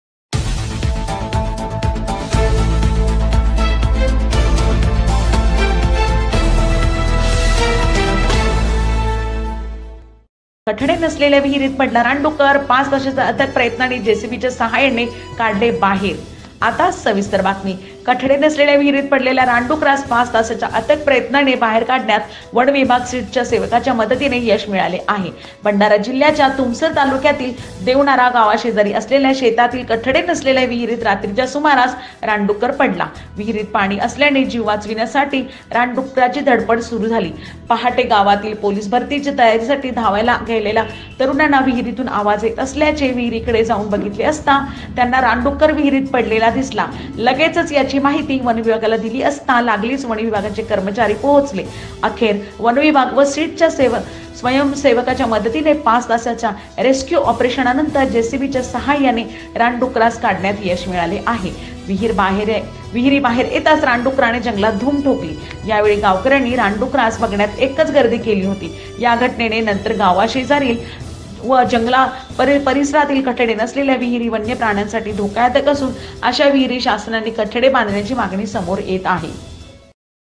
5 hours of forest section to save pig....
भंडारा जिल्ह्याच्या तुमसर तालुक्यातील देवनारा गावाशेजारी असलेल्या शेतातील कठड़े नसलेल्या विहिरित रात्रीच्या सुमारास रानडूक्कर पडला।विहिरित पानी असल्याने जीव वाचविन्यासाठी रानडूक्कराची धडपड सुरु झाली।पहाटे गावातील पोलिस भर्तीची तयारीसाठी धावायला गेलेल्या तरुणाना विहिरितून आवाज येत असल्याचे  विहिरिकडे जाऊन बघितले असता,त्यांना रानडूक्कर विहिरित पडलेला दिसला।लगेच यांची माहिती वनविभागाला दिली असता लागलीच वन विभागाचे कर्मचारि पोहचले ।अखेर वनविभाग व सीट च्या स्वयंसेवकांच्या मदतीने 5 तासाच्या रेस्क्यू ऑपरेशन नन्तर जेसीबी च्या सहाय्याने रानडूक्करास काढण्यात यश मिळाले आहे